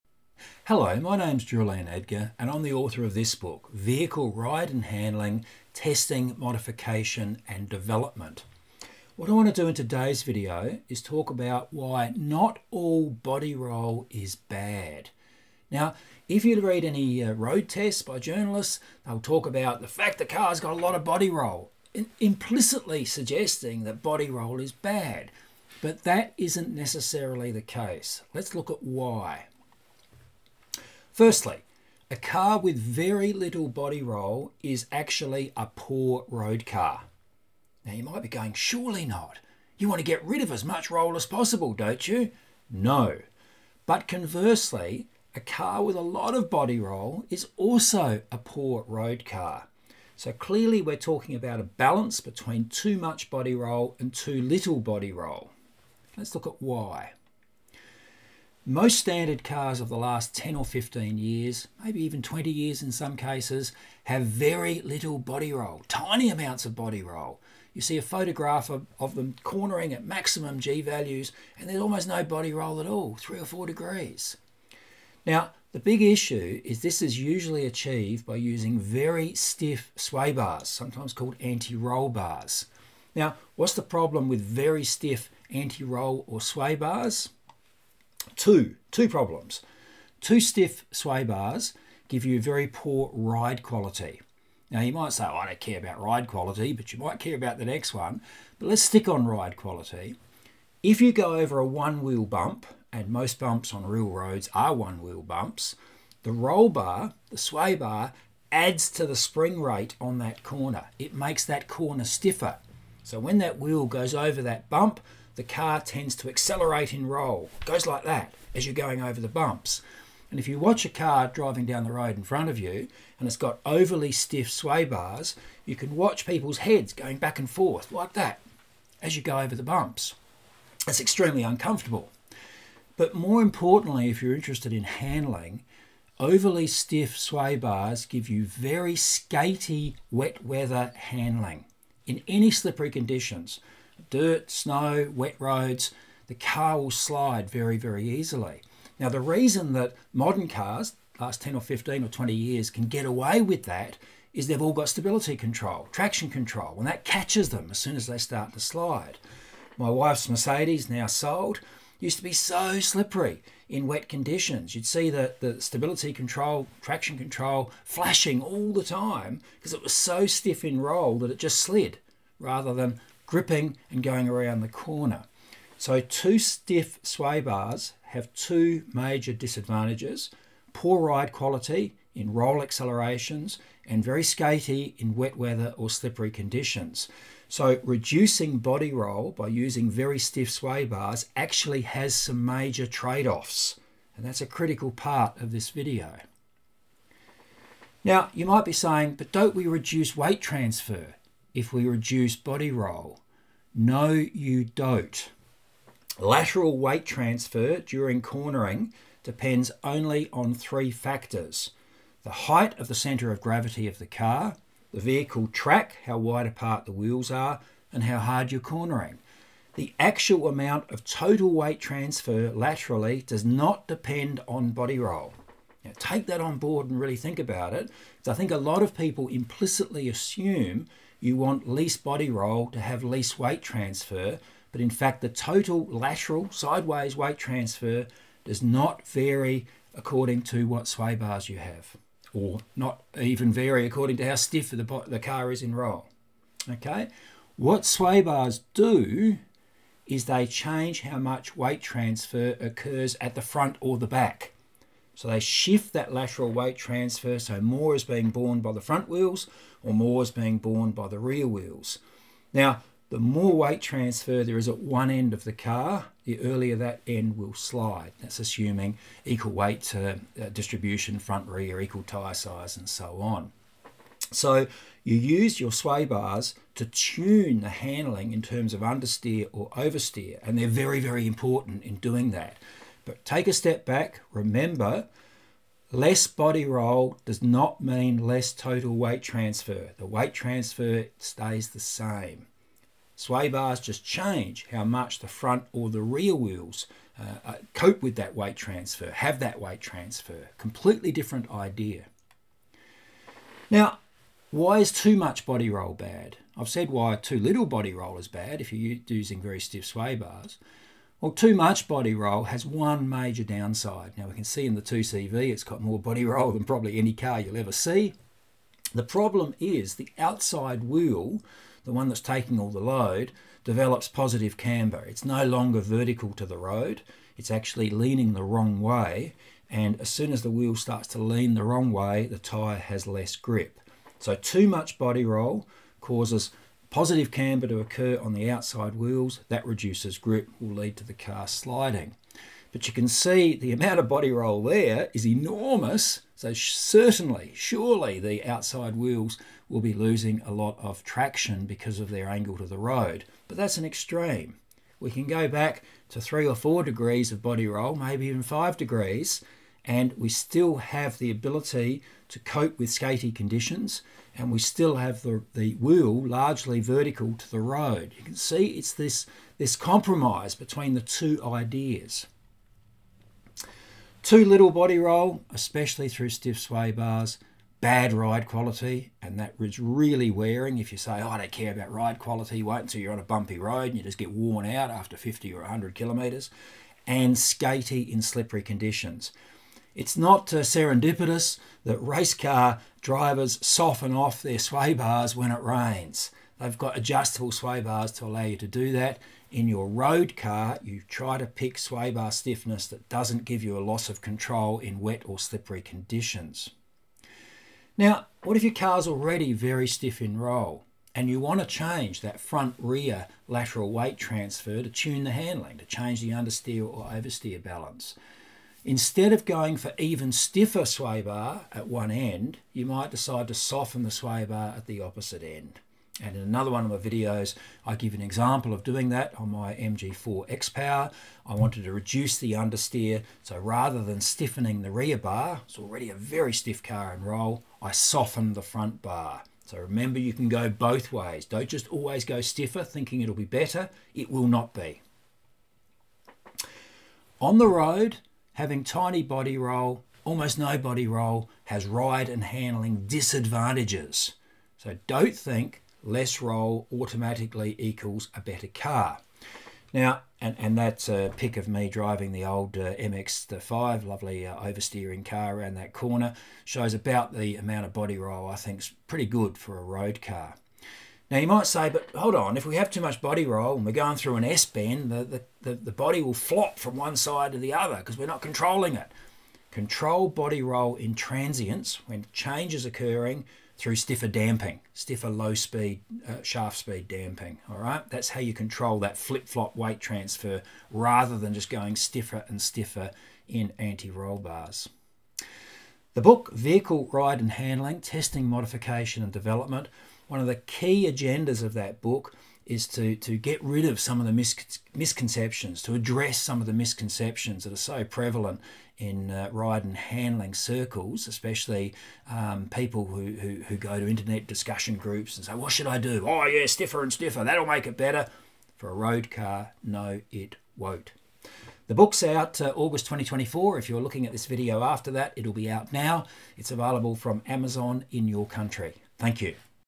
Why not all body roll is bad
People often think that on their road car, less body roll = better handling. But that often isn't the case. Here's why. or Amazon in your country.